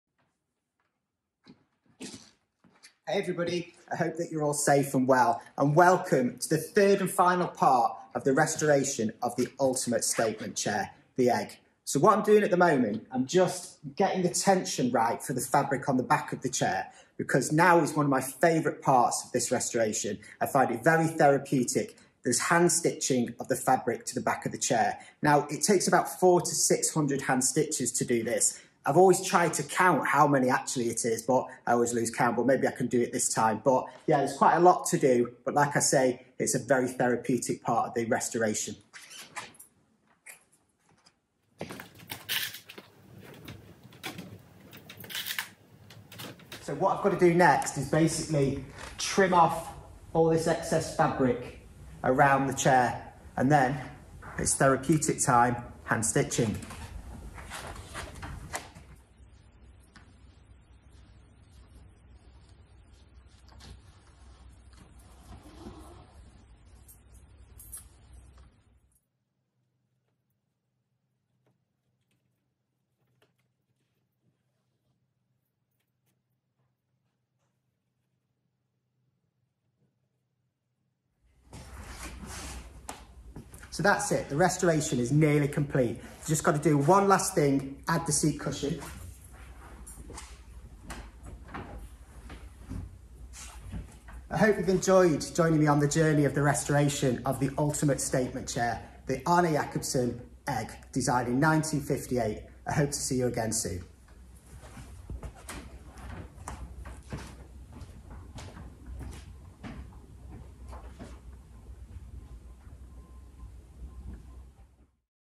Reloved Upholstery's restoration of the ultimate statement chair: the Egg™ (Part 3)
Here's part 3 of Simion's latest makeover of the ultimate statement chair, the Egg™, originally designed by Arne Jacobsen in 1958.

Really hope you enjoy the video! Don't forget to click like if you did, and look out for more upholstery inspiration coming soon.

Big thanks!